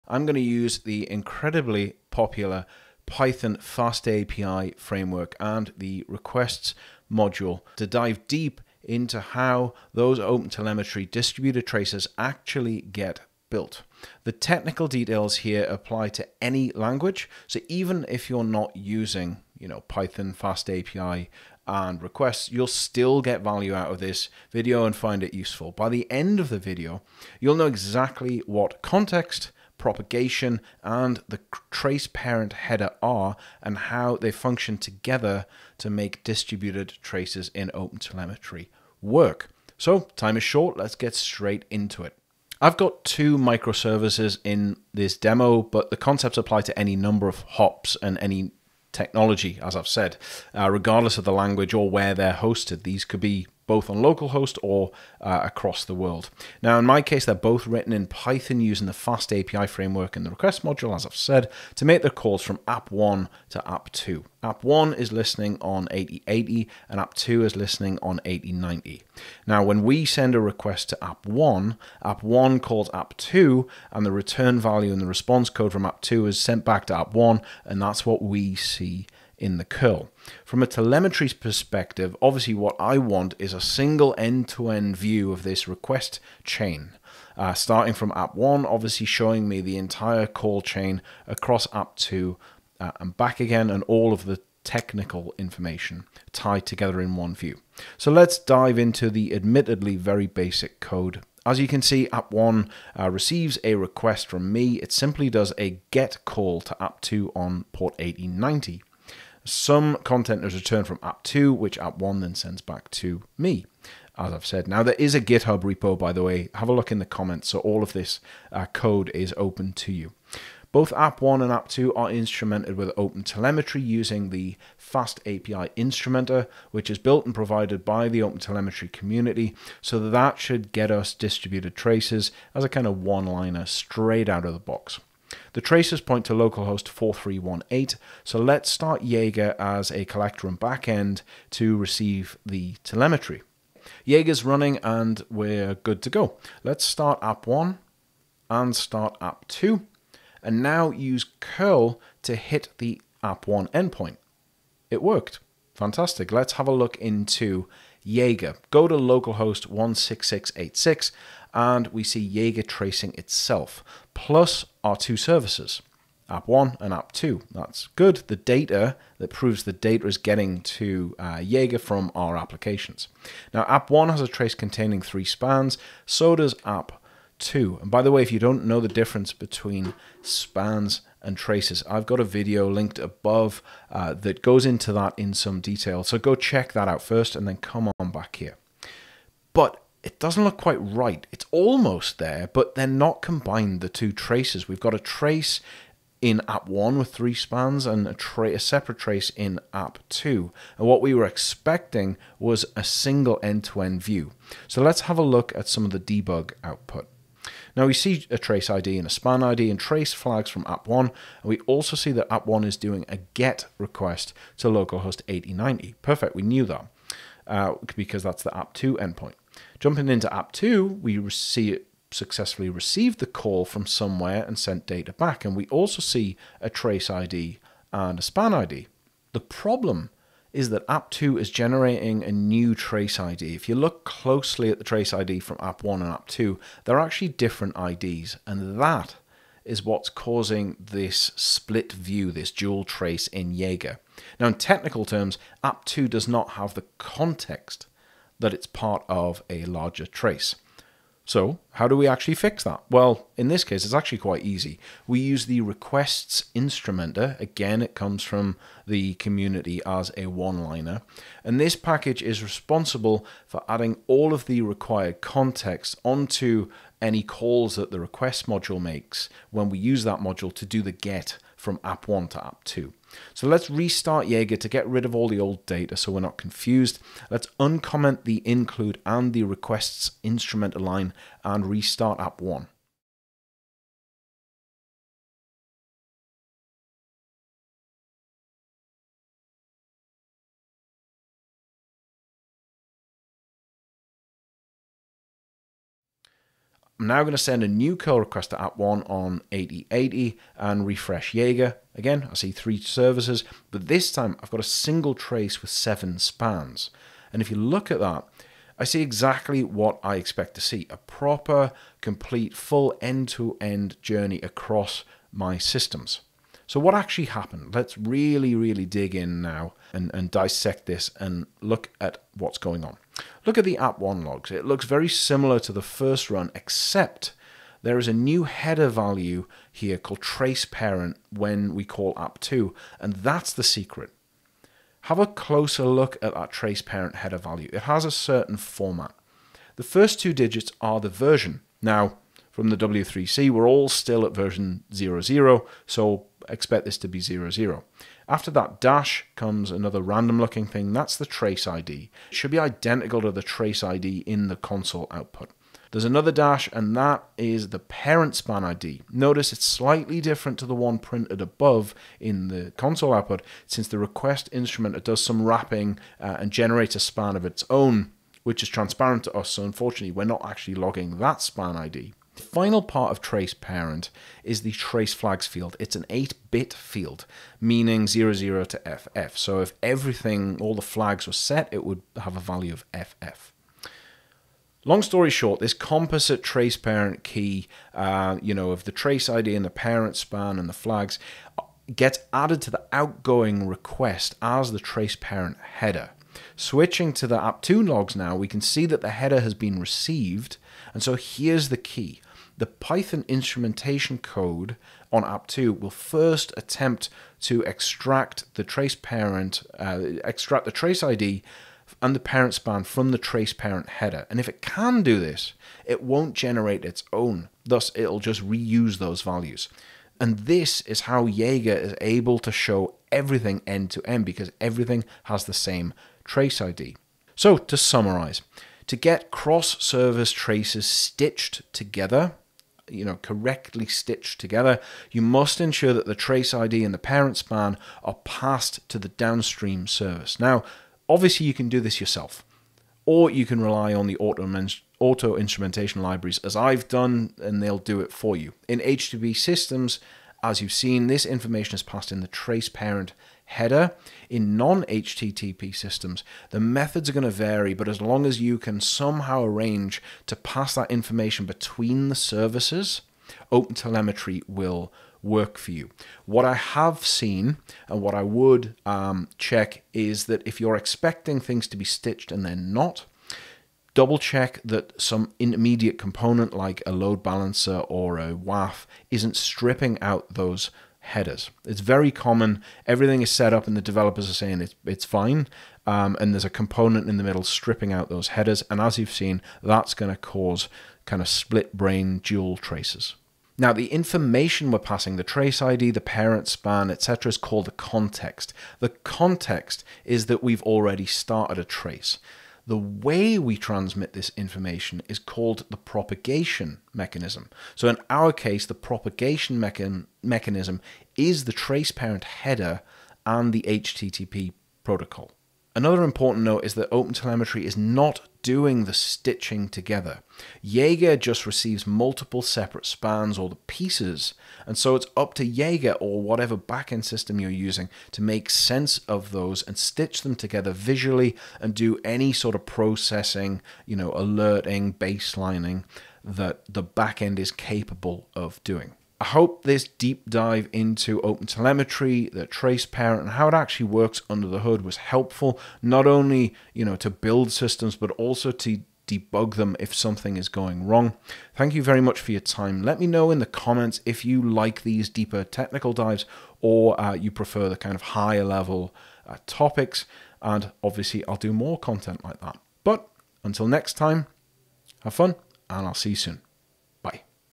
OpenTelemetry: How distributed tracing really works with Python, FastAPI and requests
A technical deep dive explanation of how OpenTelemetry distributed tracing really works under the hood with examples using Python, FastAPI and the requests module and Jaeger. I discuss the traceparent header and its importance in correctly stitching a trace together end-to-end.